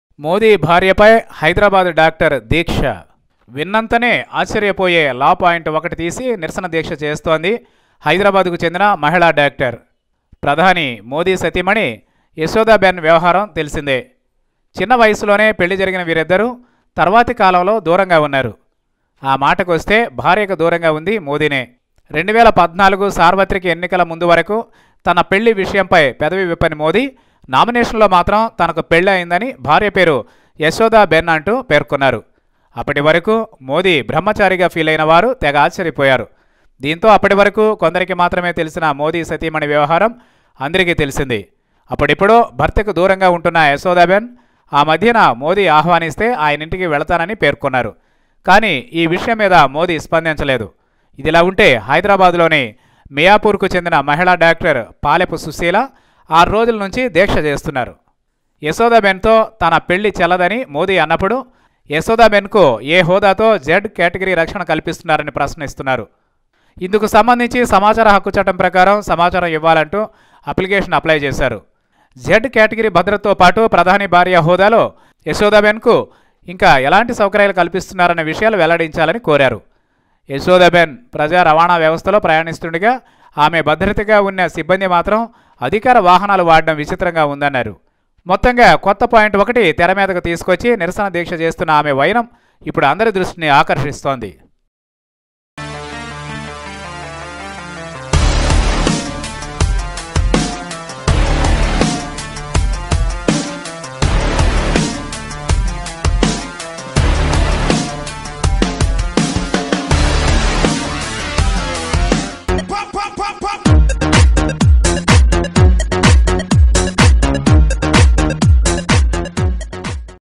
ప్రధాని మోడీ భార్యకు భద్రతపై మహిళా డాక్టర్ దీక్ష..!| Hyd Lady Doctor Protests Against Modi's Wife..!
Hyd Lady Doctor Protests Against Modi's Wife..!
Hyderabad -based woman doctor Palepu Susheela has been protesting since six days regarding this issue. She questioned, 'How can Yashodaben receive Z-Category Security when Modi declared his Marriage with her isn't valid? On what basis, She was provided the security cover? An RTI Petition was moved seeking information on Yashodaben's security and other facilities she has been enjoying. When Yashodaben travels in public transport, The security personnel follows her in private vehicles. The whole set-up appears very surprising'.